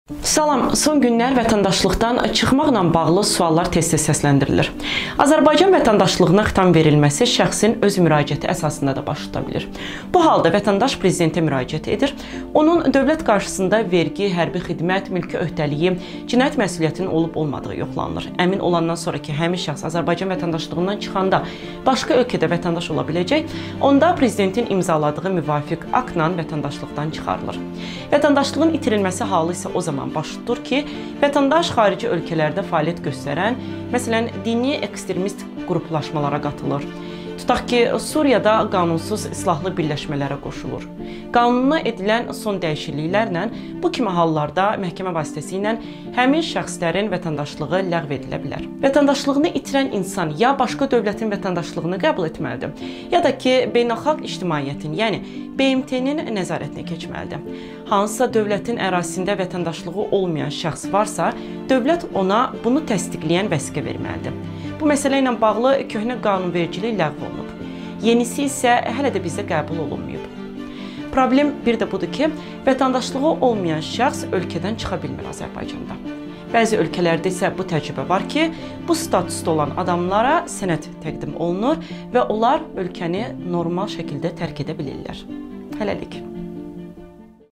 Komment: Vətəndaşlıqdan çıxmaq və çıxarılmaq
Son günlər vətəndaşlıqdan çıxmaqla bağlı xeyli suallar ünvanlanır. Vətəndaşlıqdan çıxmaq və çıxarılmaq: bu proseslər necə aparılır?  Vətəndaşlığı olmayan şəxs Azərbaycanı tərk edə bilirmi? İzah edirik.